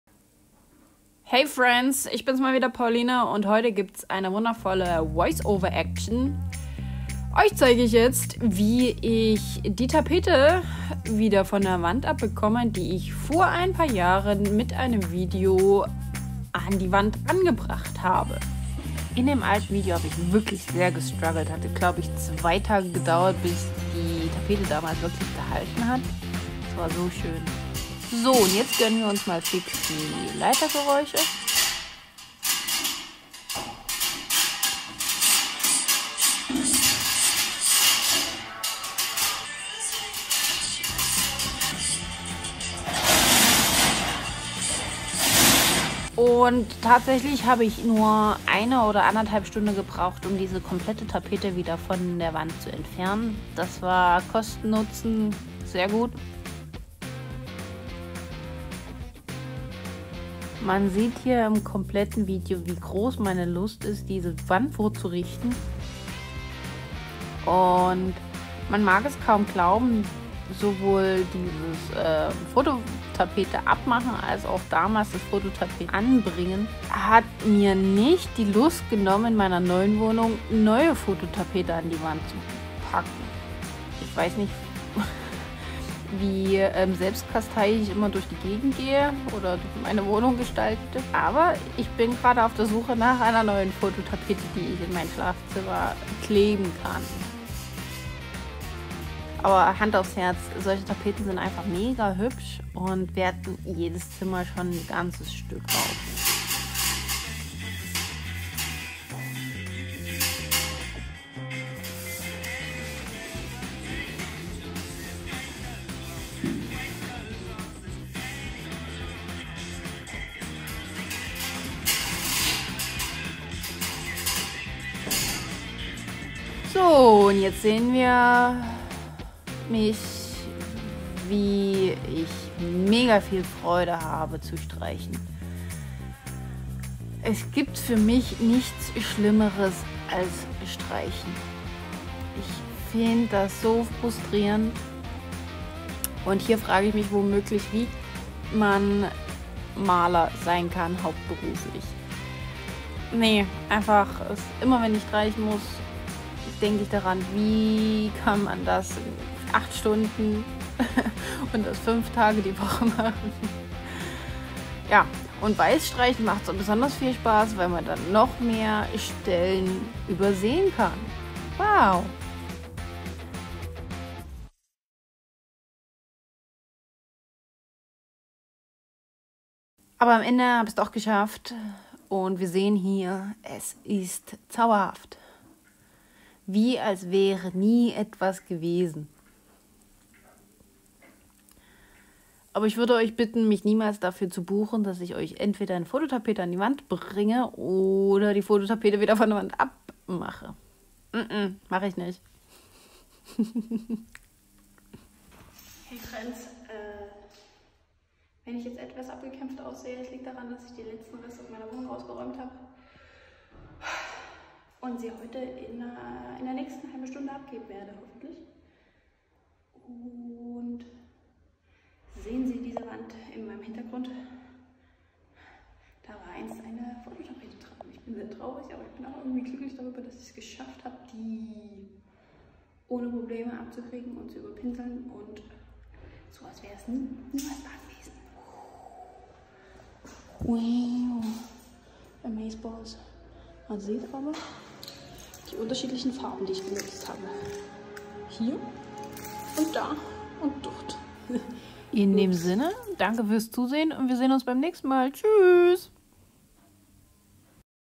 Good Bye Fantasy Forrest - ich entferne meine Komar Fototapete
Erinnert ihr euch noch an das Video, wie ich damals diese Tapete an die Wand gebracht habe?

Jetzt bin ich aus der Wohnung ausgezogen und musste die Fototapete wieder entfernen. Zu meinem Erstaunen hat das mega gut und einfach funktioniert und mittlerweile habe ich die alte Wohnung auch schon erfolgreich abgeben können.

Freut euch schon auf das nächste Fototapeten Video 🎉🎉🎉